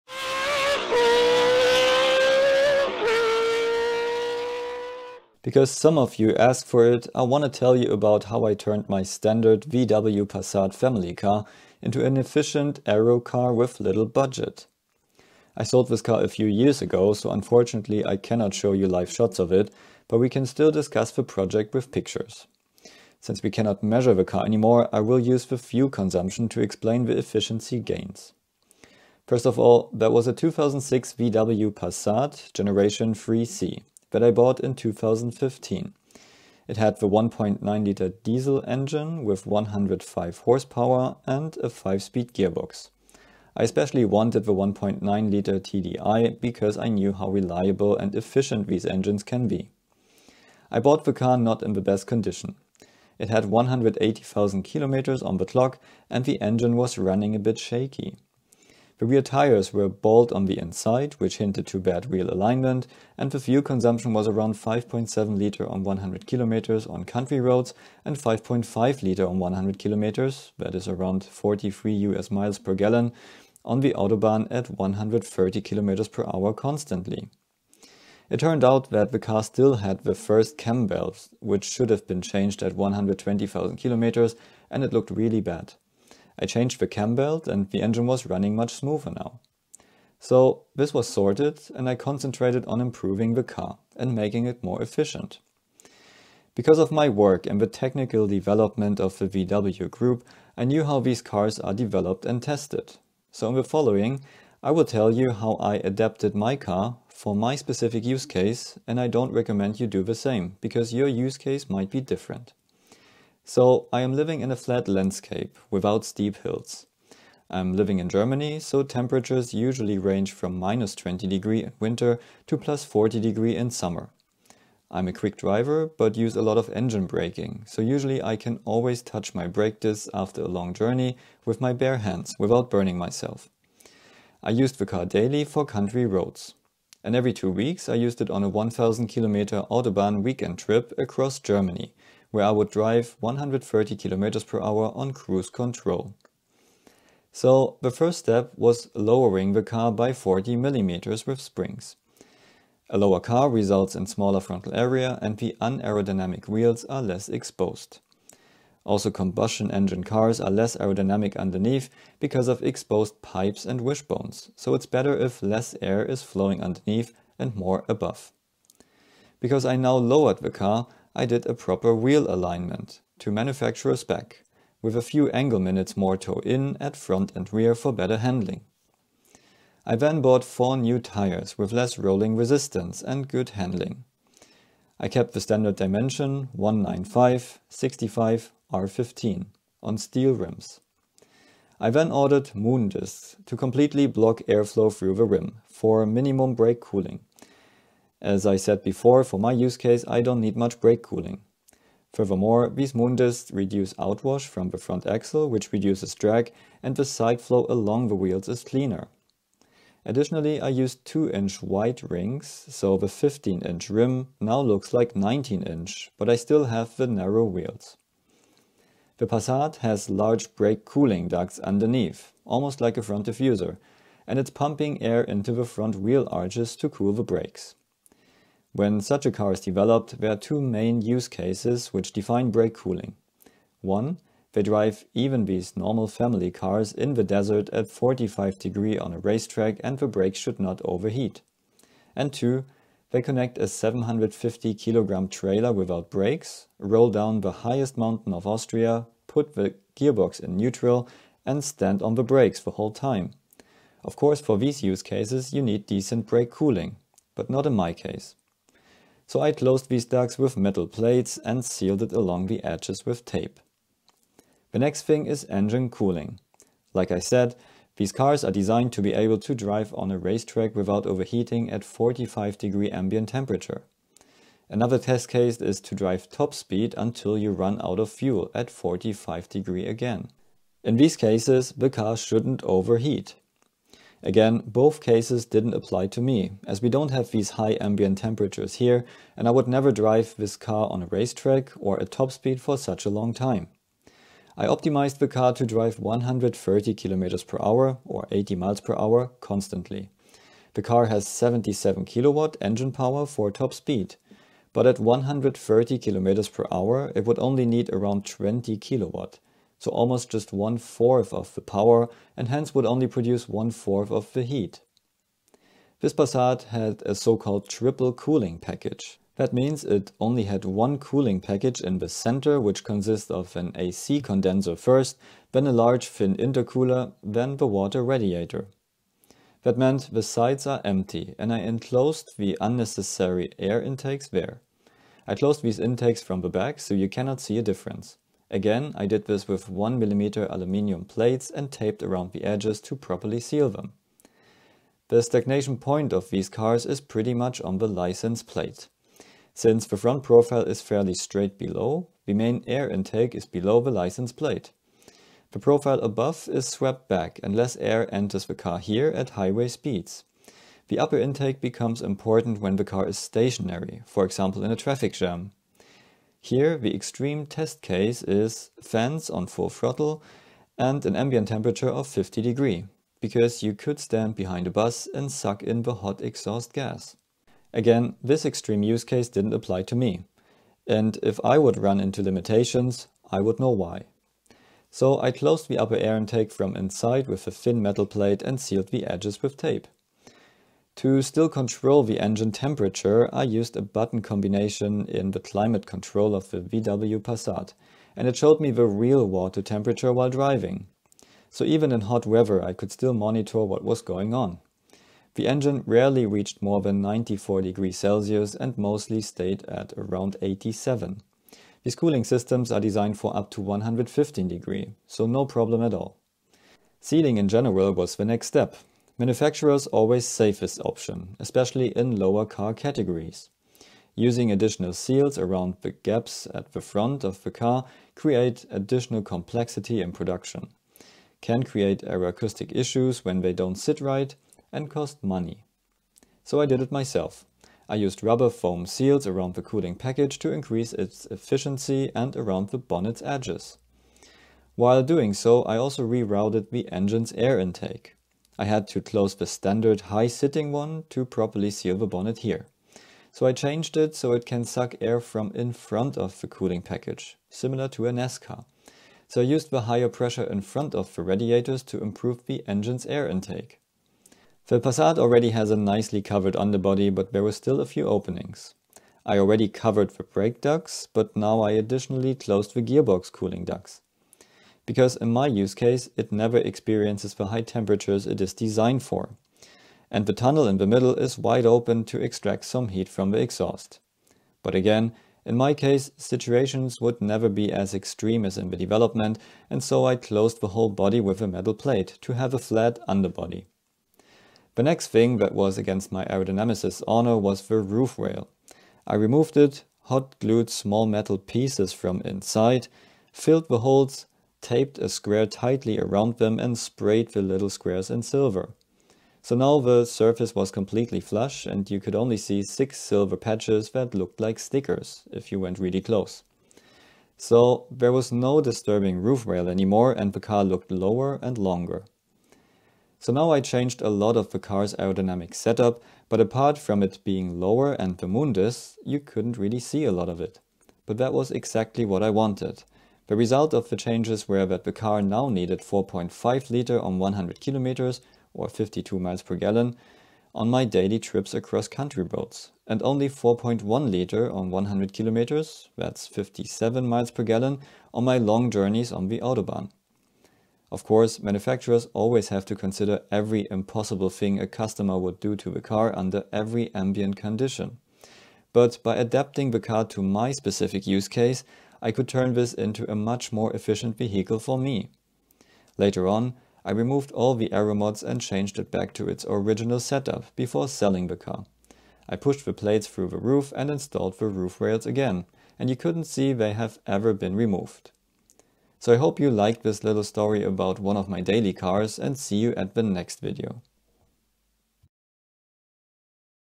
Massive Drag Reduction For Tiny Budget - How I turned my family car into an AERO Star
Let's have a closer look at how I improved aerodynamics on my family car a couple of years back.

How are these cars developed?
Why is there so much potential for efficiency gains?
And how did the efficiency of my car change after the modifications?

Costs of the project:
5000EUR     Car
300EUR       Tires
200EUR       Moon Discs
100EUR       Lowering Springs
20EUR         Foam Seal
10EUR         Aluminium Plates
5EUR           Tape
5EUR           Spray/Filler